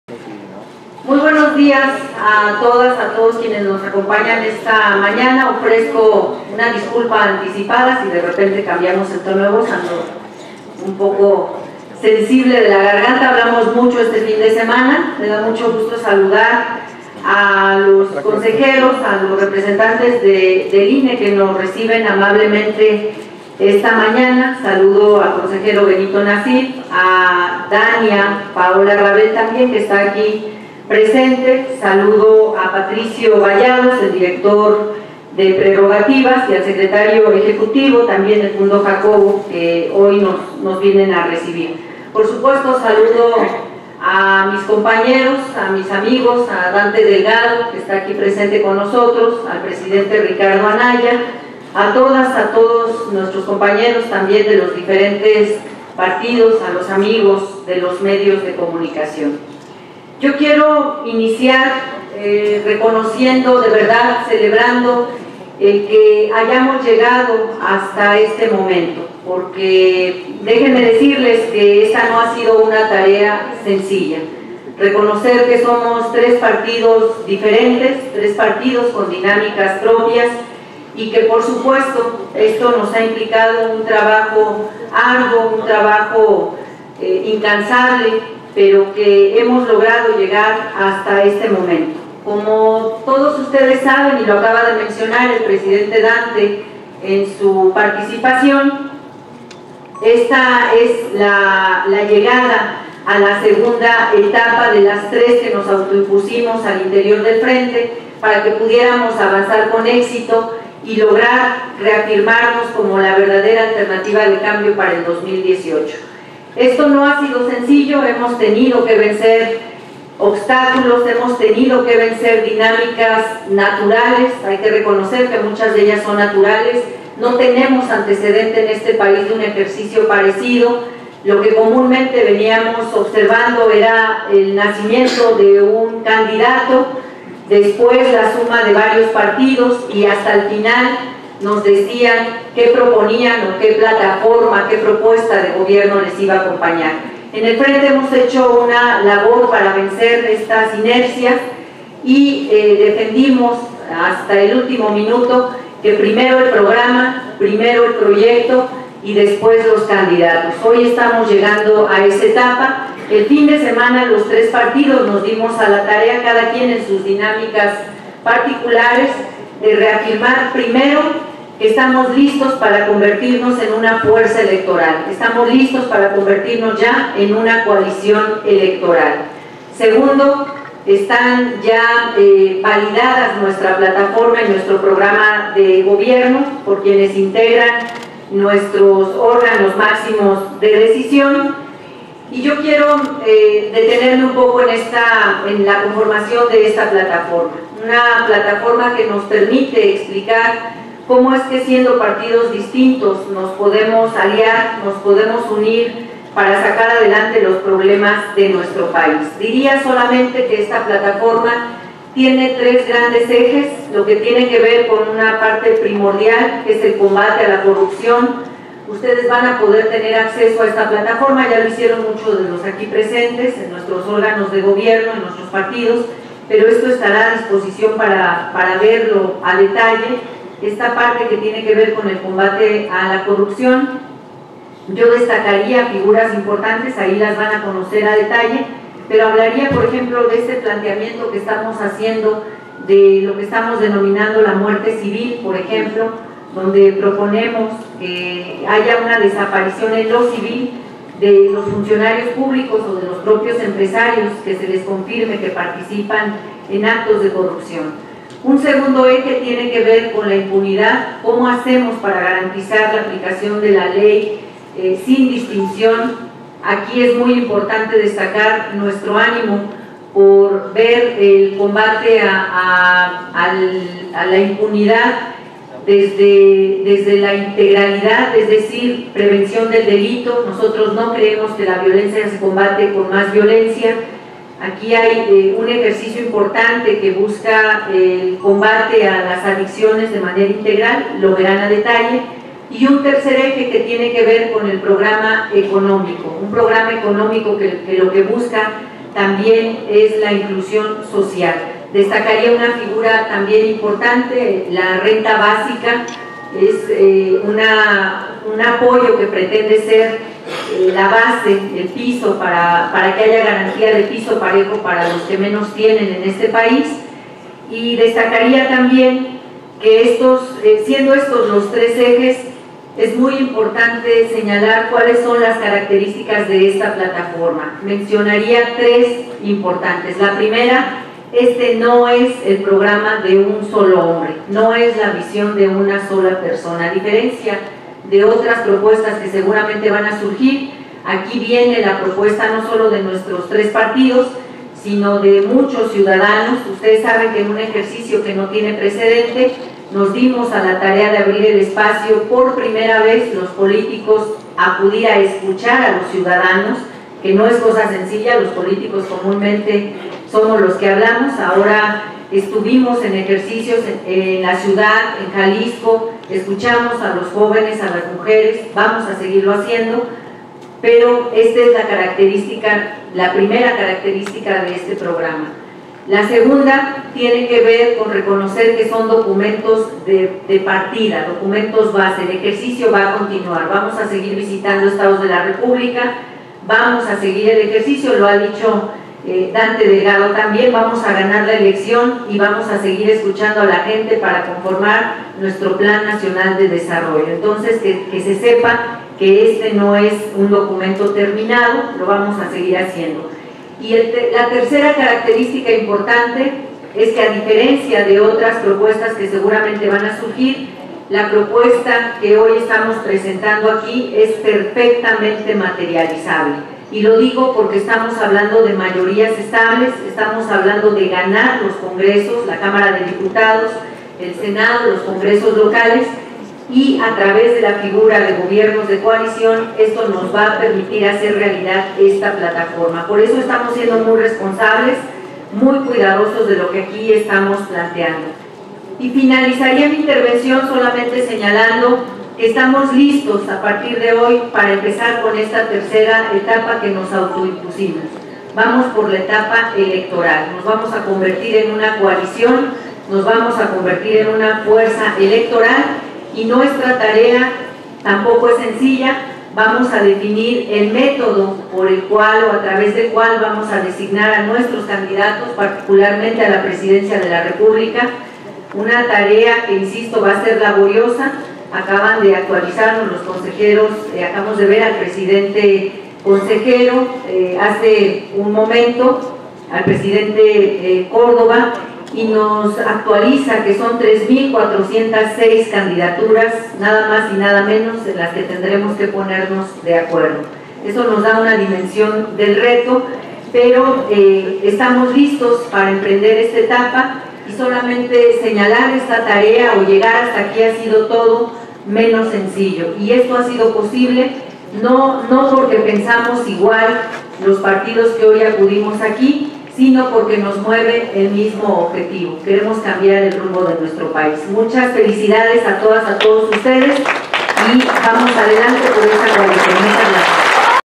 Entrega de convenio de coalición ante el INE del FrenteCiudadano.
Los que mueve al Frente Ciudadano  son las causas y nos anhelos de  los mexicanos.